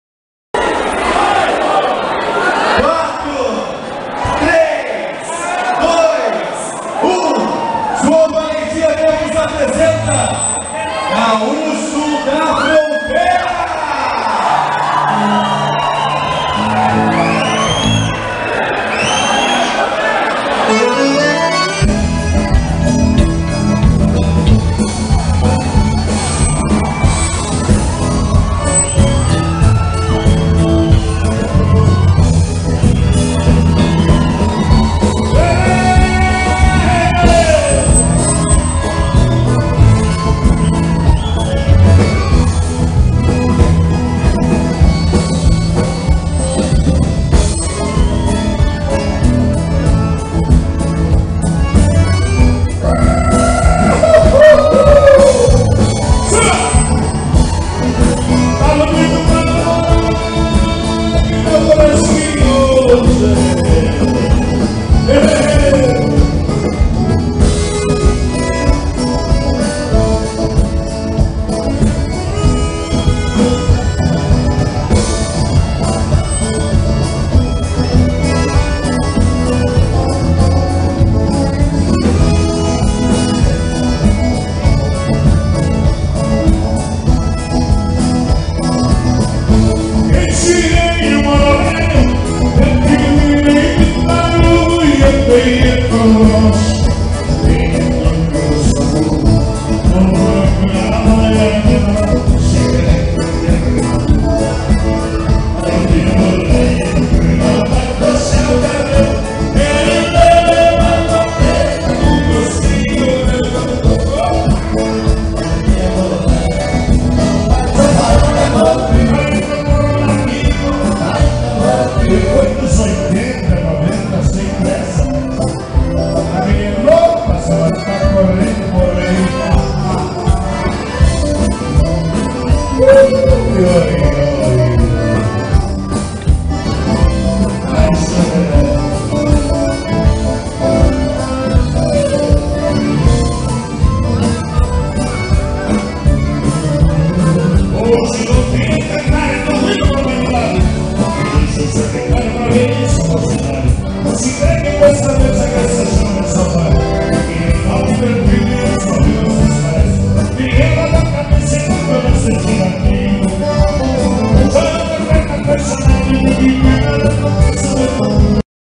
Canal 500 Notícias Gaúcho da Fronteira ao VIVO Show ao vivo com Gaúcho da Fronteira
Gaúcho da Fronteira ao VIVO em Borrazópolis